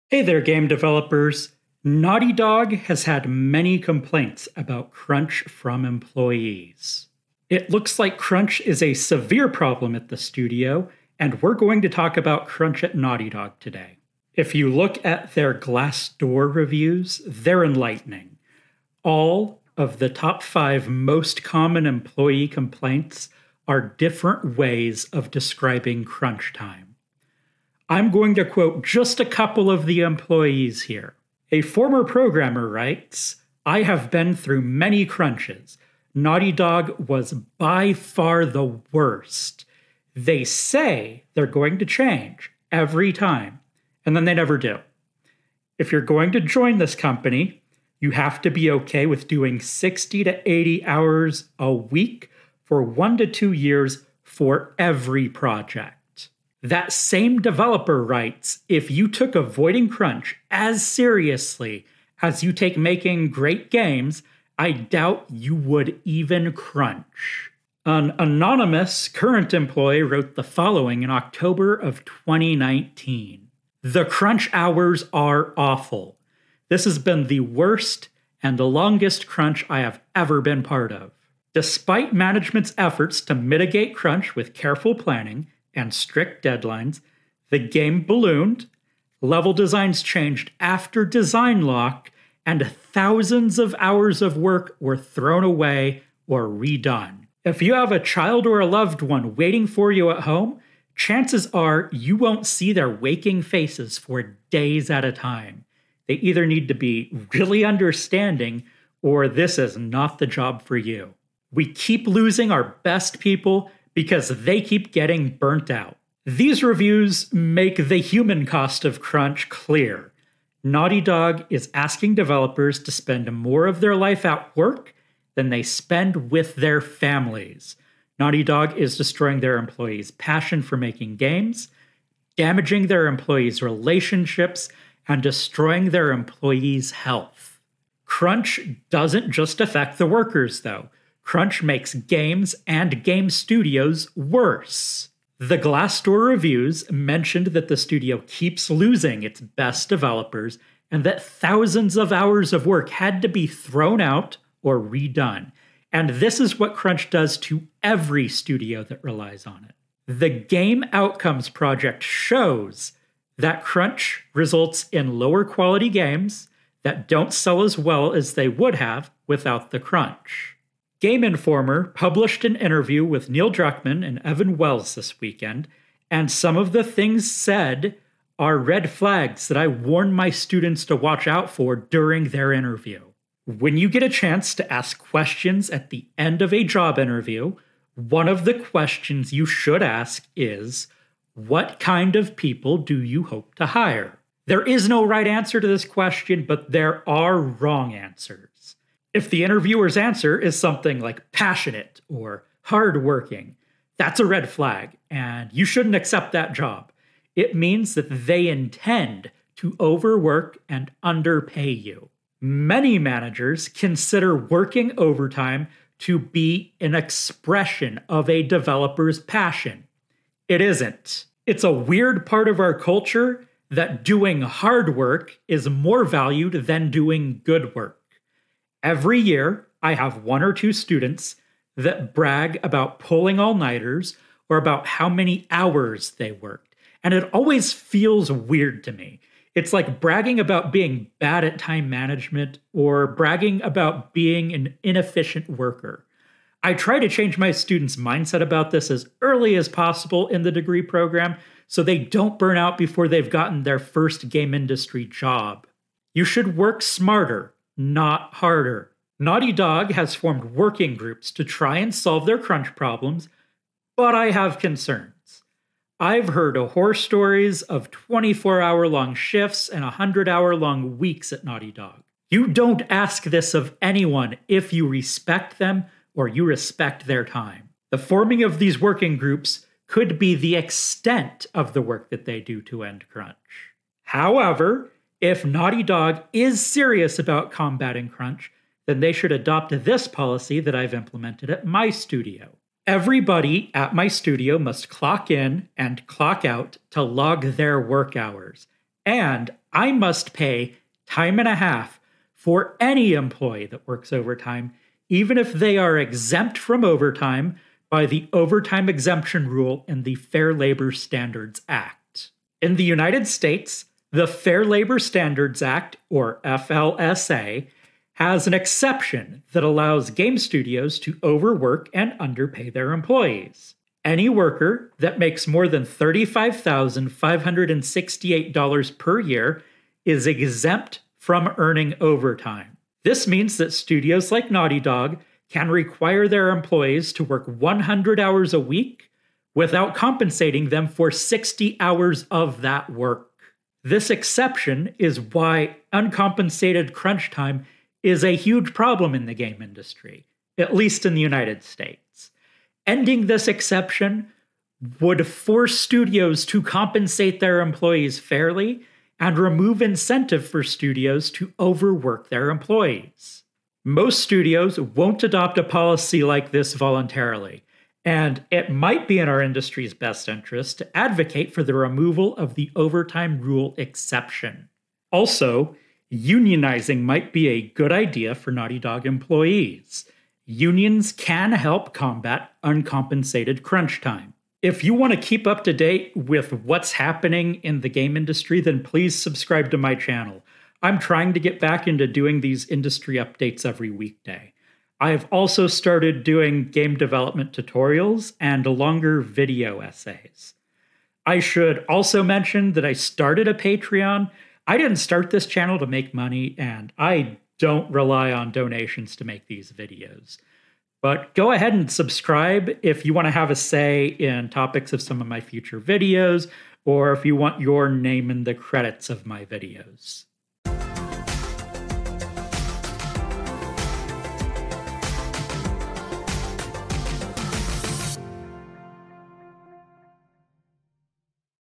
Combating Crunch Culture at Naughty Dog
Naughty Dog has a severe problem with crunch culture in their studio.

Let's talk about this problem and why crunch culture is so damaging.

While we're at it, let's talk a bit about how labor laws in the United States allow studios to exploit game developers, and what game developers can do to fight back against this exploitation.